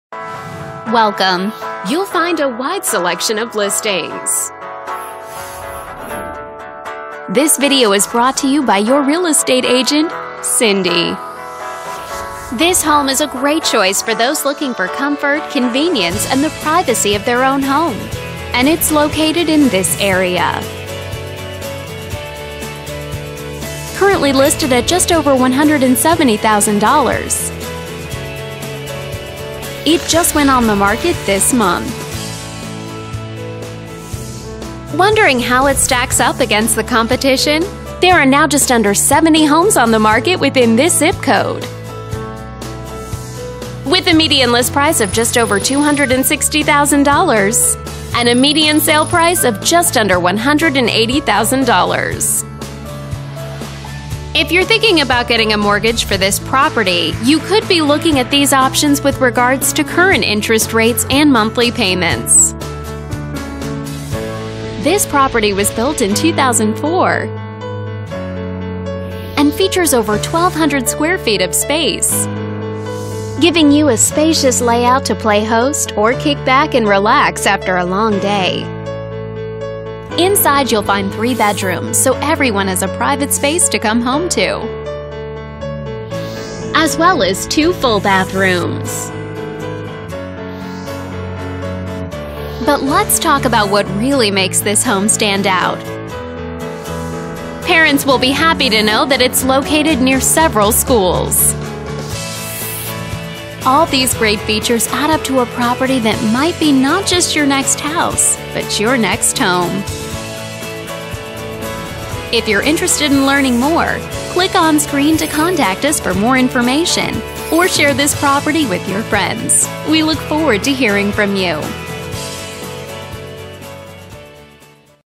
Homes for Sale in Grand Junction 2669 Sheene Road
$179,900 and CUTE, CUTE, CUTE! Relax by your gas fireplace as you gaze at your Mountain Views!!! Or play in the 1/3 ACRE truly park-like backyard w/a Magic Treehouse climbing tree and lots of amazing space for baseball games & summer BBQ's!! GIANT Monster-size RV Parking for lots of BIG TOYS!!! NEWER finishes & wonderful colors, LARGE laundry room, outstanding shelving in garage, and beautiful PRIDE IN OWNERSHIP throughout!!! 3/2/2 in an outstanding LOCATION and Views!!!